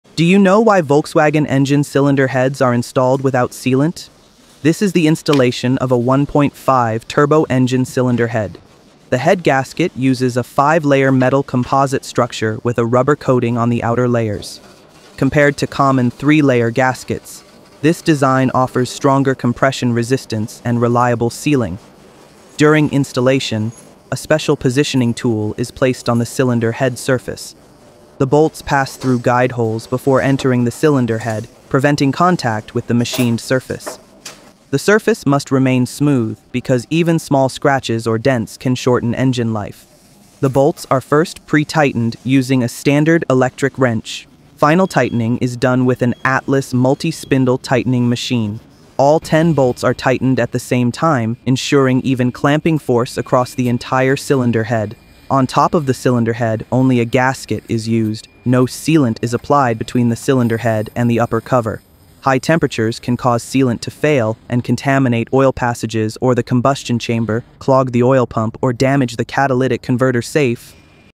Why Volkswagen Engines Skip Sealant on Cylinder Heads
Learn why Volkswagen engines, including the Jetta one point five turbo, never use sealant on cylinder heads. We break down the gasket design, bolt installation process, and why using sealant can actually harm your engine.